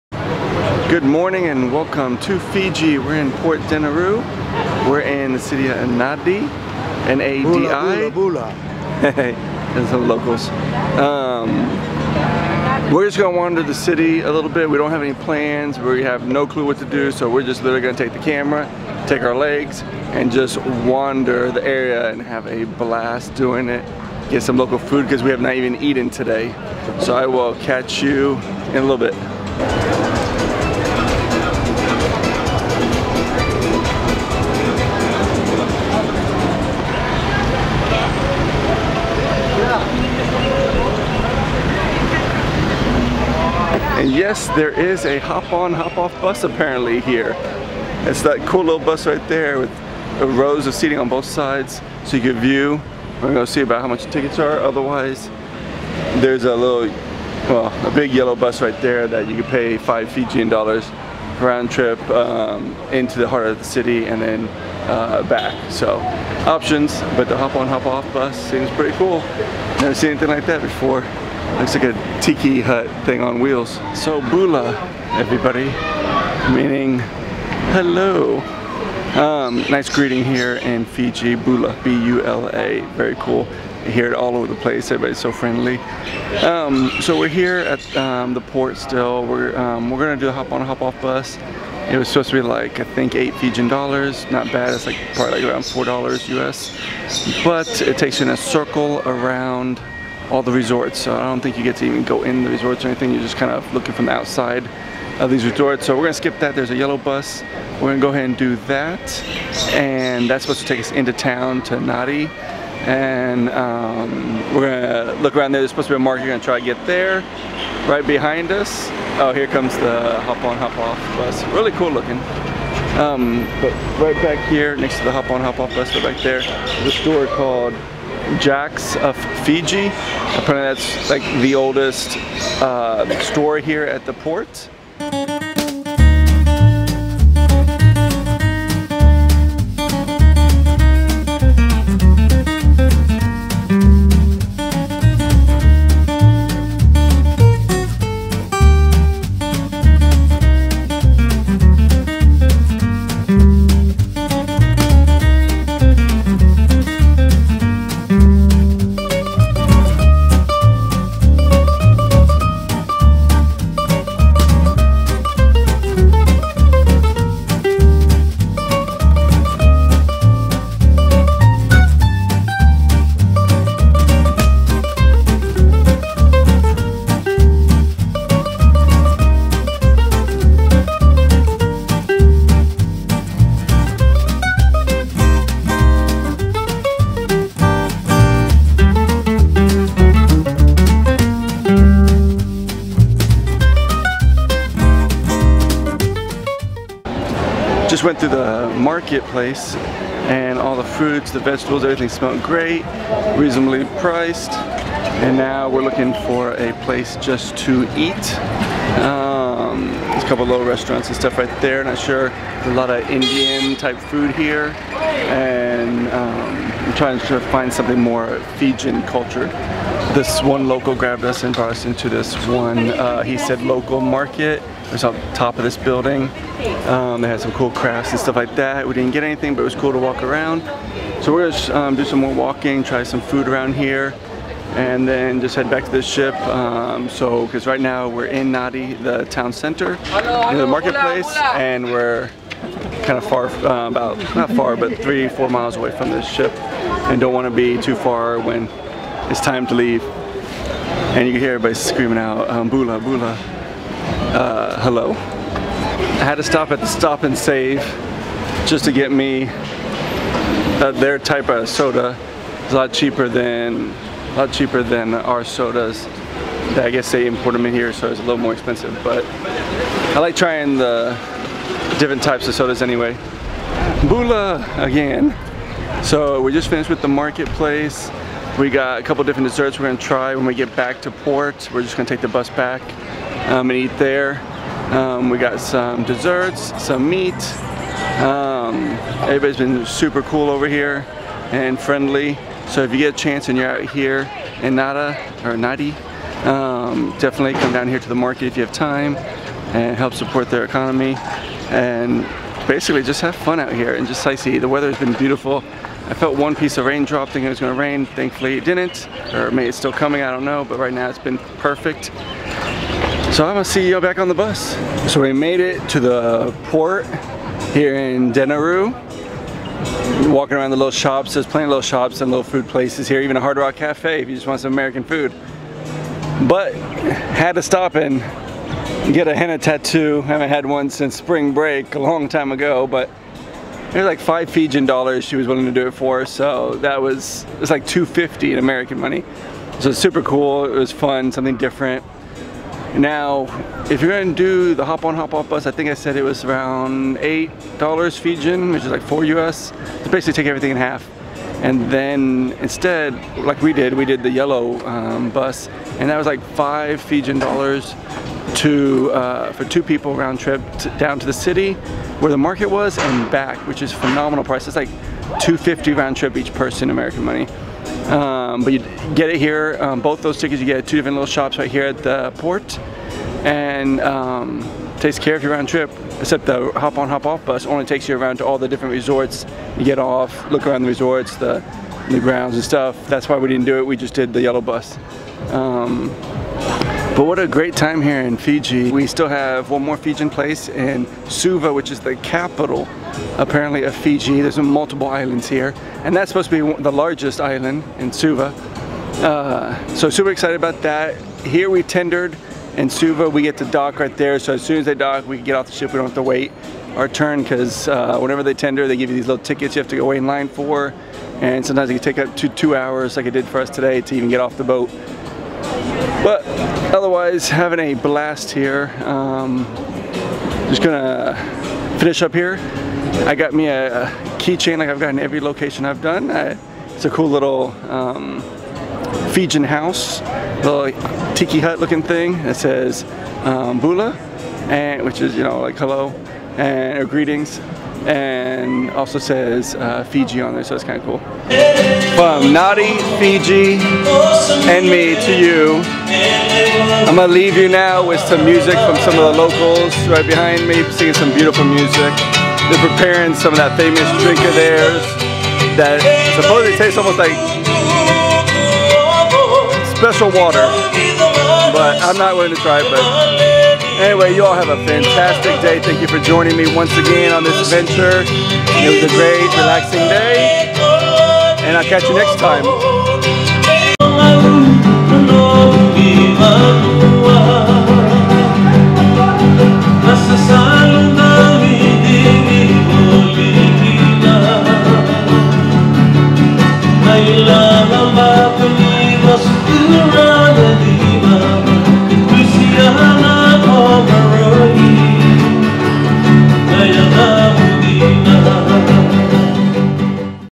Visiting Port Denerau (Nadi), Fiji (Cruise Vacation Stop 37)
Today is a relax day and I head into town to experience some Fijian culture and check out the markets and try some of the local food. End the day with a Hentai tattoo. Here are my impressions of my first time in Fiji.

Join me on my travels as i put down the stethoscope and experience life.